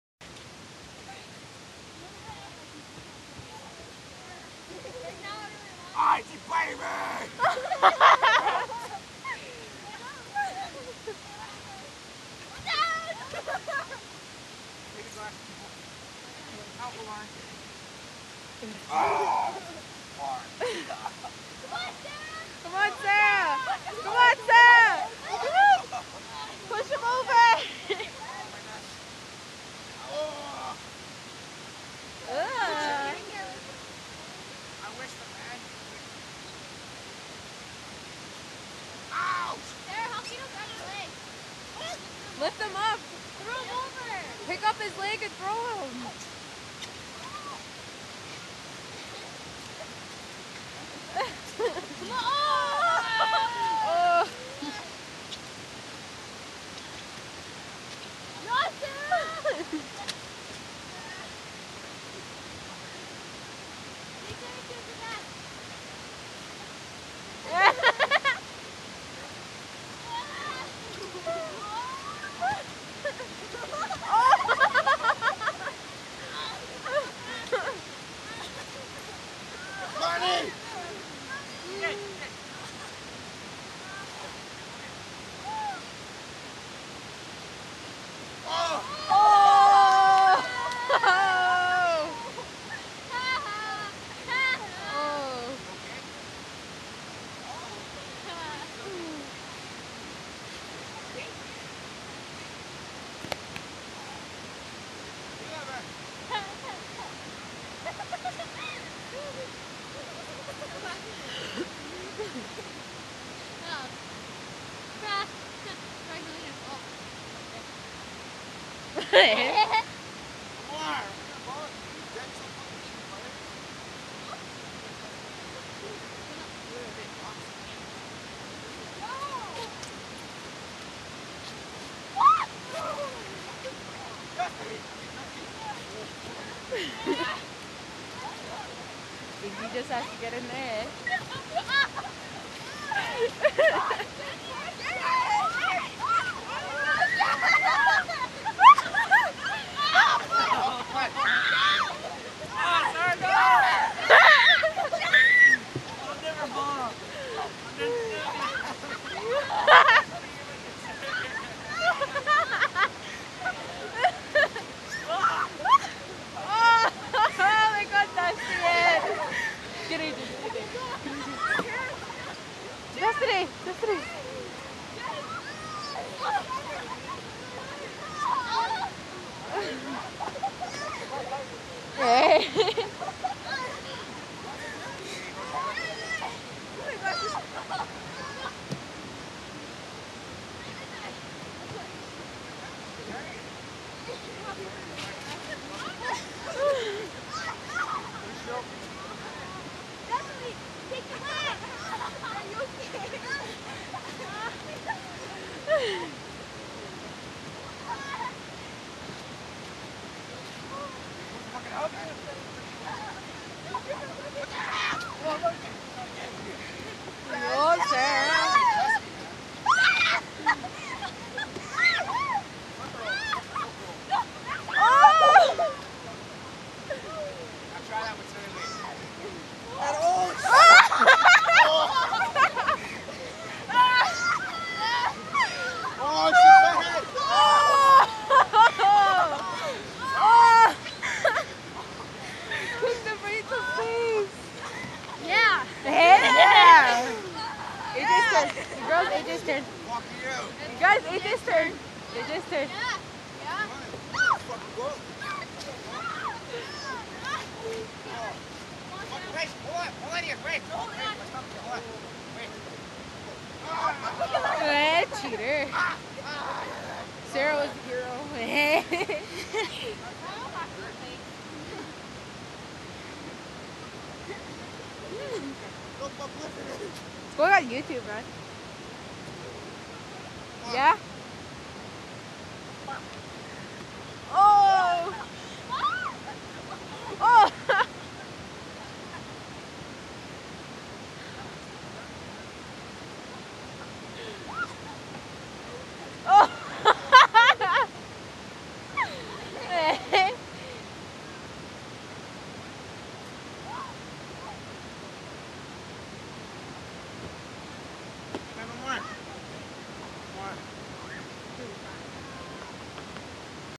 play fighting
Haha, This is My friends Ajay, Sara and Jenn and some of my familt, dusty, destiny, iggy, and my other little buddies from my rez play fighting outside of the school!